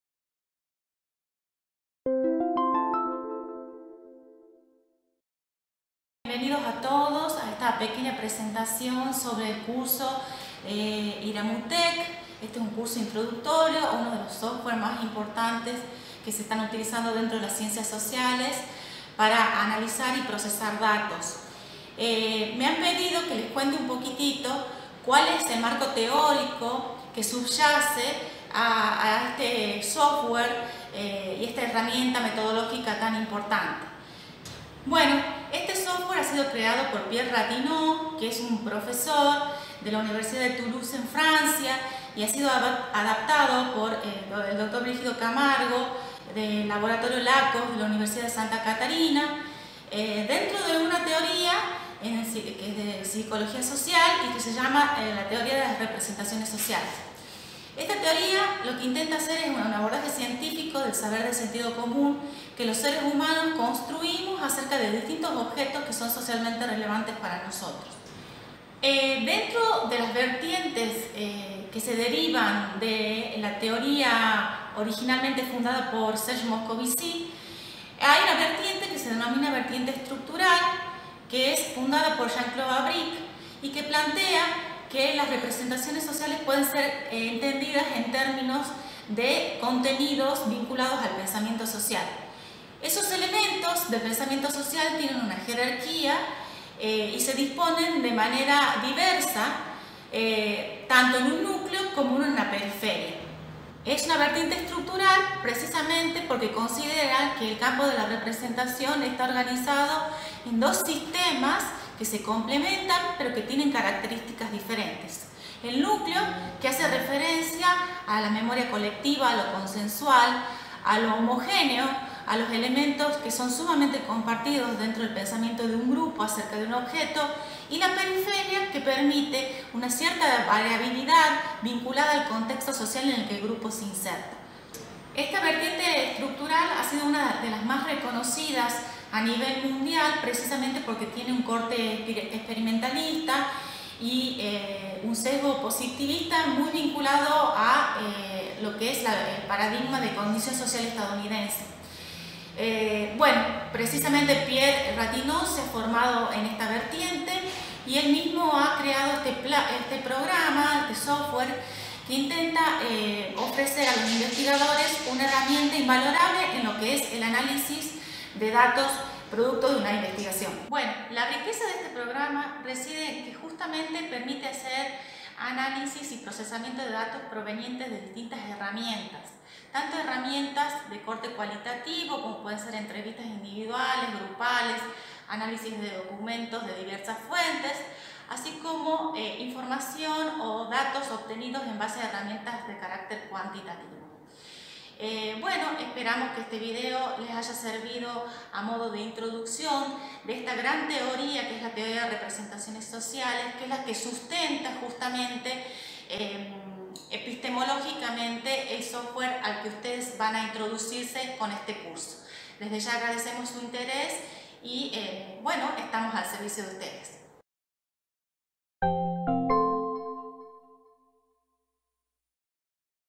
Introducción al Software Iramuteq
Breve presentación sobre el software Iramuteq, contexto en que surge, teoría que subyace, tipos de análisis que permite realizar.